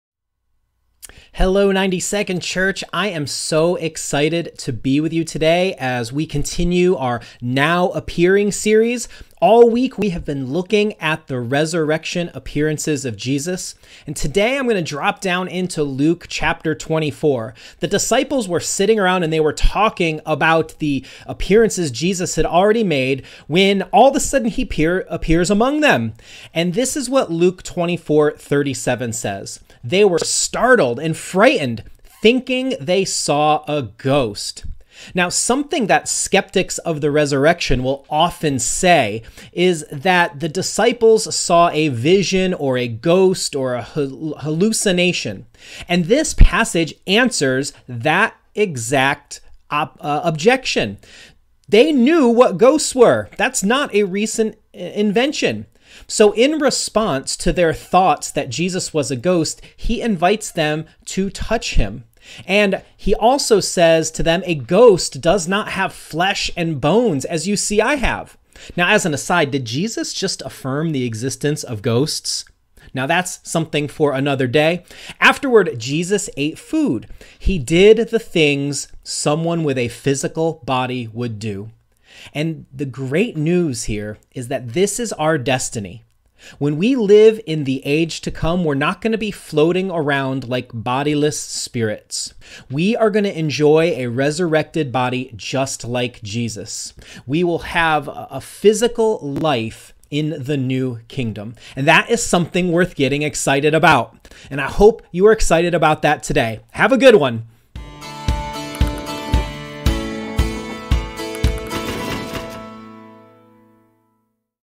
90SecondChurch: Thurs, Apr 8
Does Jesus affirm the existence of ghosts? What does that question have to do with the resurrection?